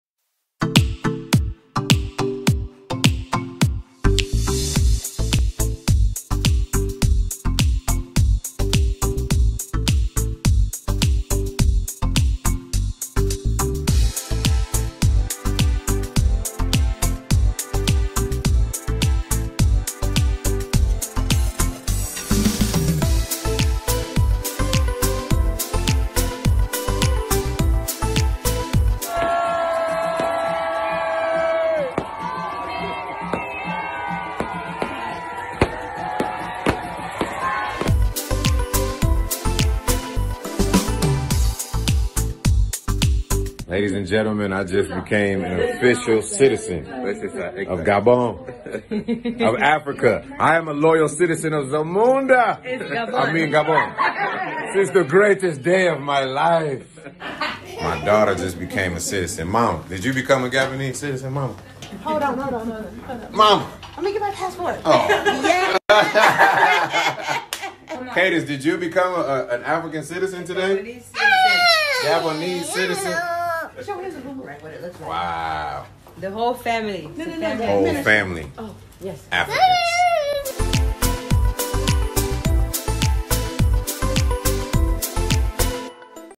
Ludacris obtient la citoyenneté gabonaise 🇬🇦
Le rappeur Américain Ludacris est naturalisé gabonais 🇬🇦